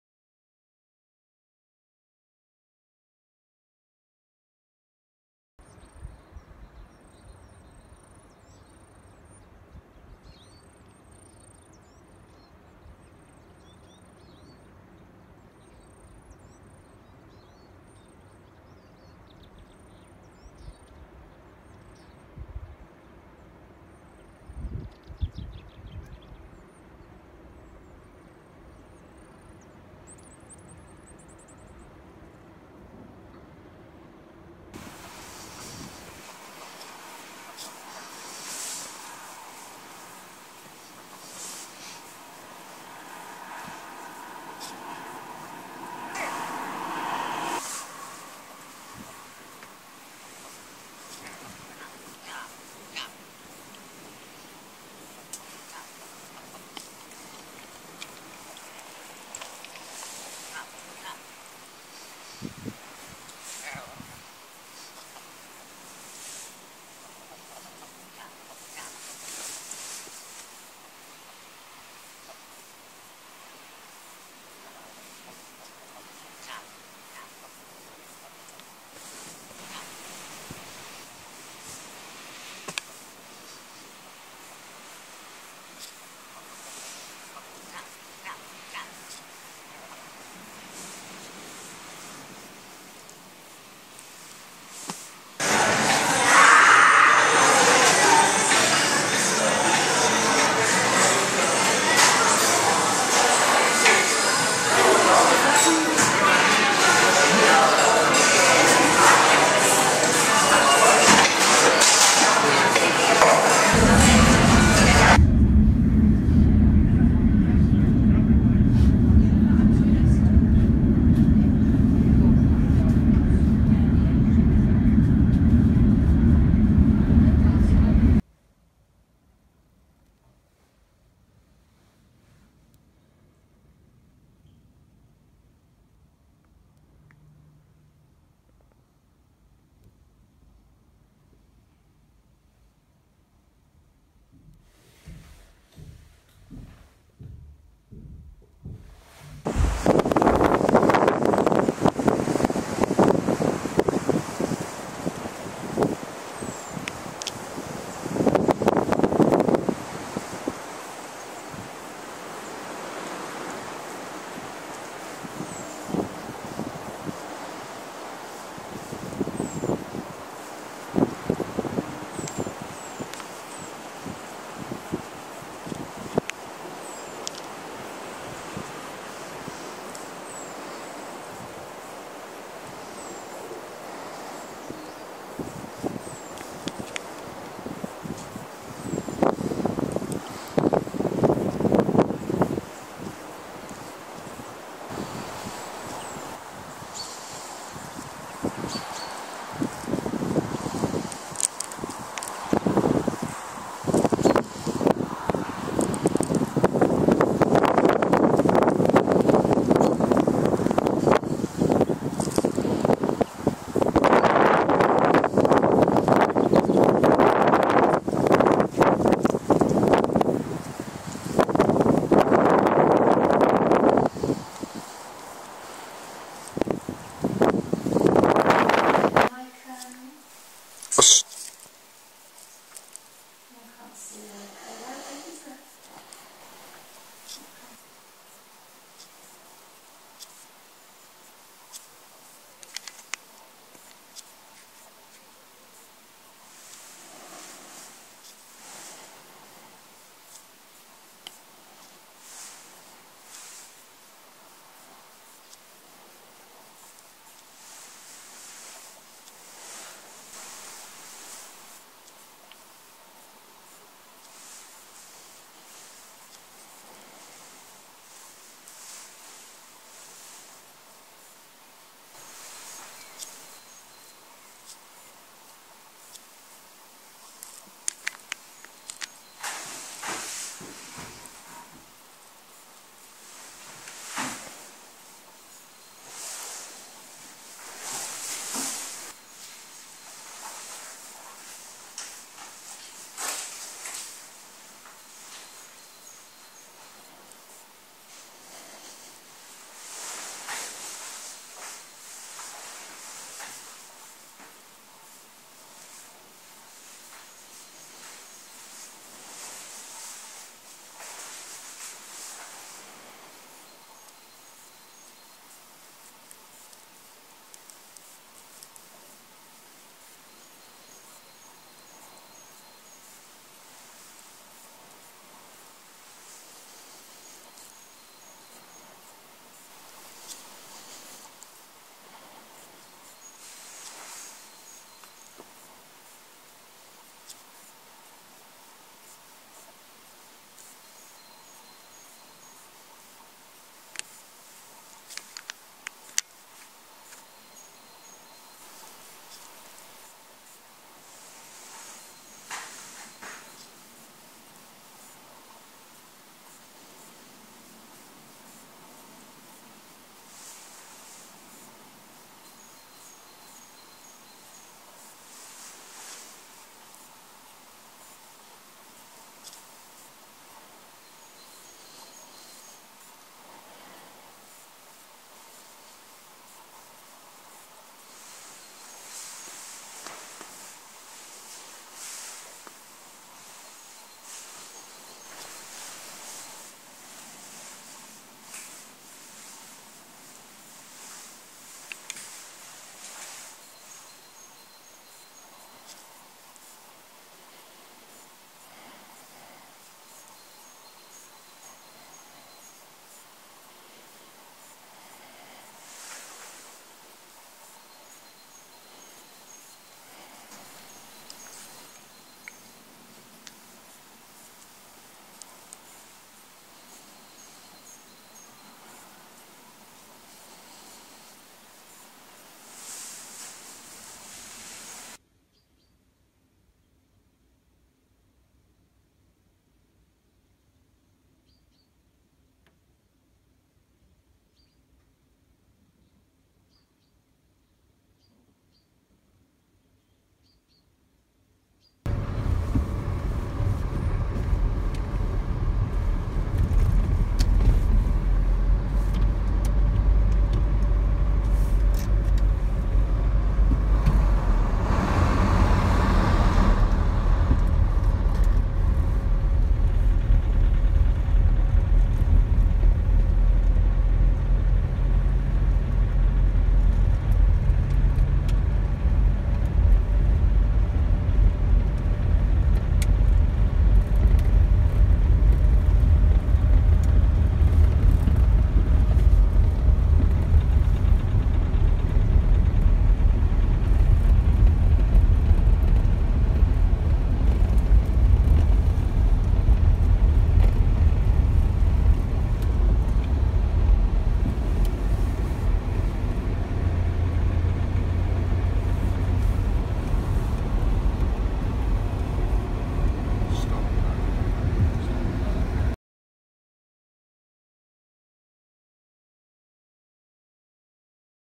Cairngorms Wildlife February 2017
A short video of a recent holiday in The Cairngorms, showing some of the wildlife we saw, including Red Grouse, Roe Deer, Stoat, Siskins and Snow Bunting